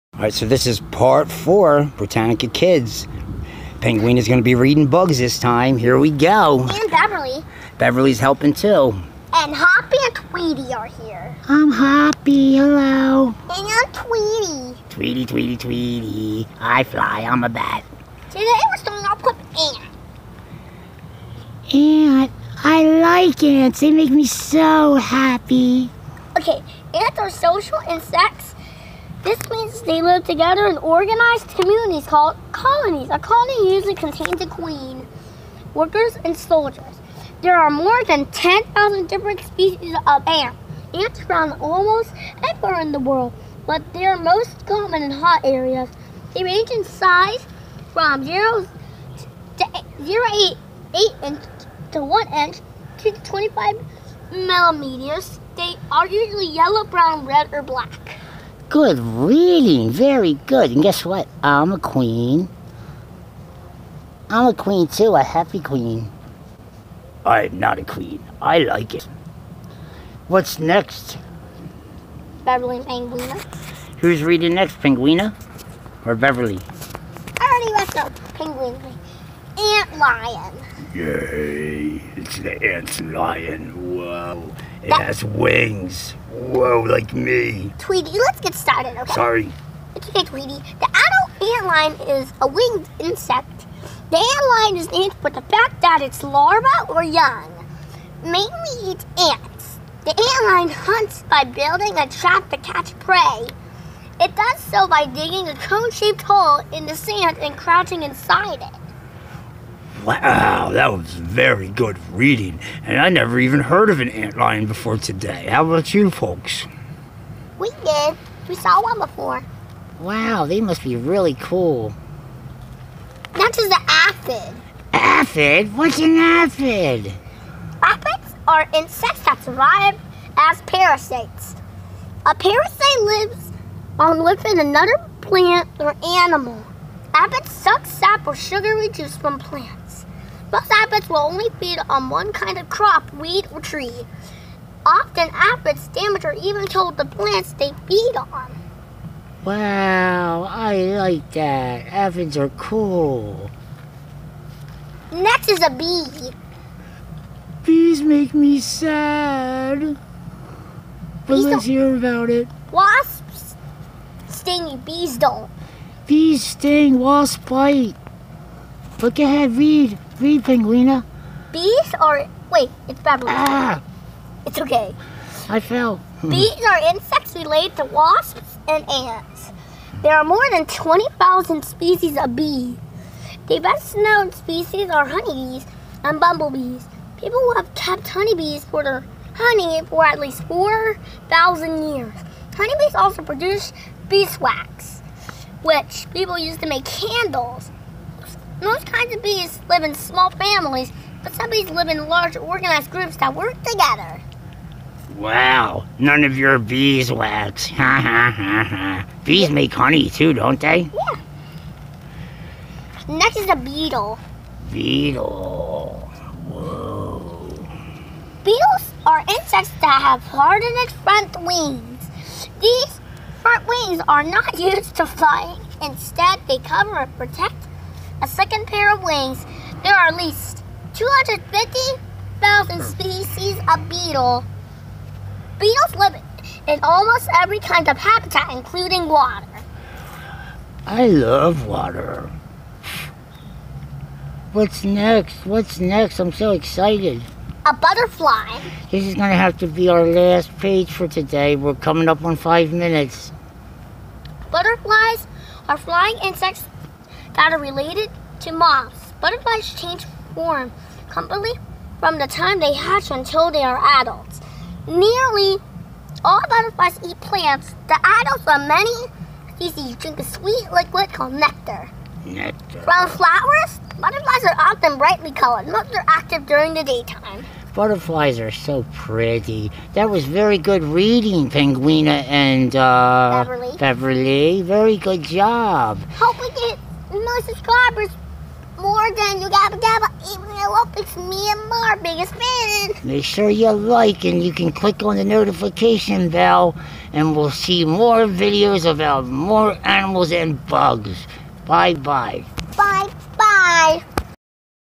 Bugs Brittanica for kids and insects fun learning
Joy, Sadness, Rainbow dash, Ronald MCD, hoppy, Tweety, Dumbo, and more. Reading by Penguina and Beverly